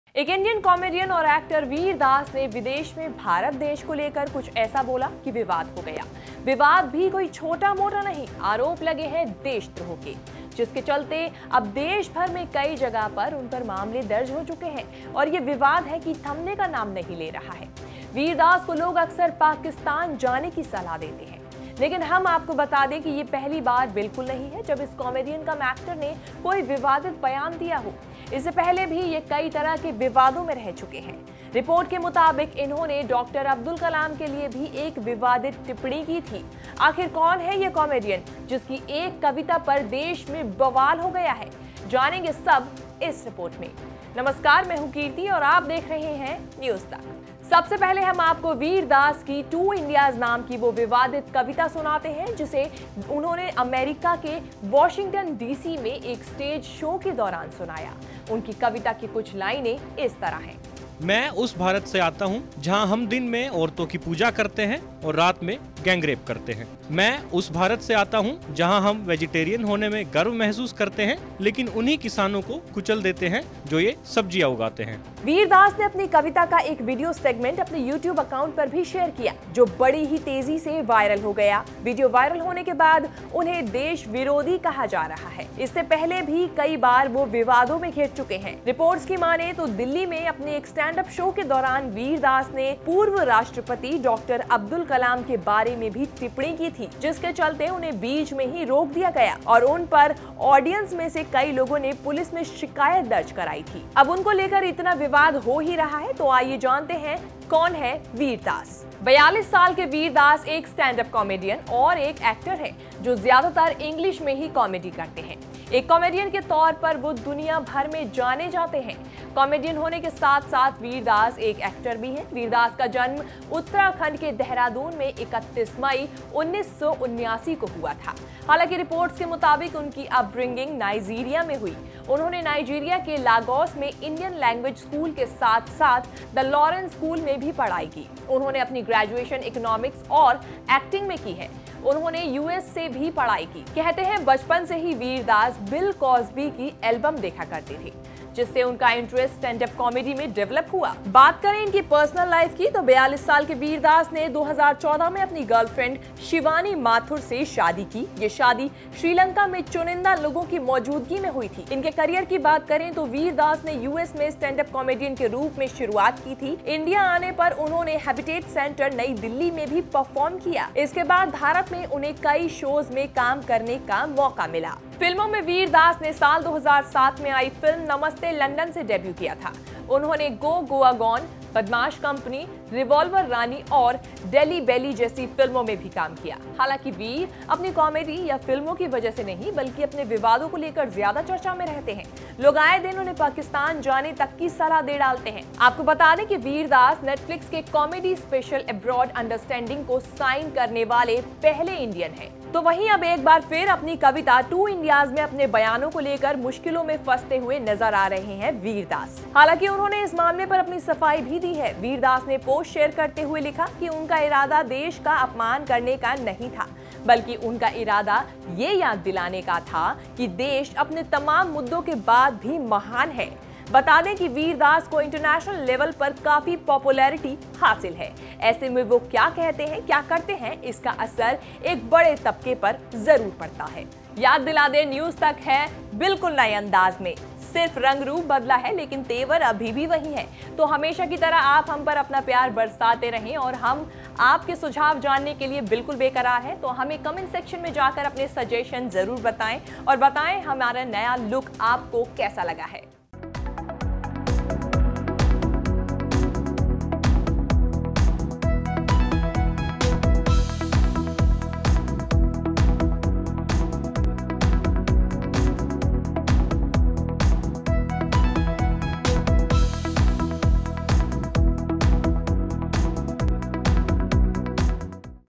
Vir Das ने Dr. Abdul Kalam पर बोला तो रोक दिया था प्रोग्राम, अब 'कविता' पर घमासान!
एक इंडियन कॉमेडियन और एक्टर वीर दास ने विदेश में भारत देश को लेकर कुछ ऐसा बोला कि विवाद हो गया... विवाद भी कोई छोटा-मोटा नहीं, आरोप लगे हैं देश-द्रोह के... जिसके चलते अब देश भर में कई जगह उनपर कई मामले दर्ज हो चुके हैं... वीर दास को लोग अक्सर पाकिस्तान जाने की सलाह देते हैं... इससे पहले भी ये कई तरह के विवादों में रह चुके हैं... रिपोर्ट के मुताबिक इन्होंने डॉ. अब्दुल कलाम के लिए भी एक विवादित टिप्पणी की थी... आखिर कौन है ये कॉमेडियन जिसकी एक कविता पर देश में बवाल हो गया है...जानेंगे इस रिपोर्ट में...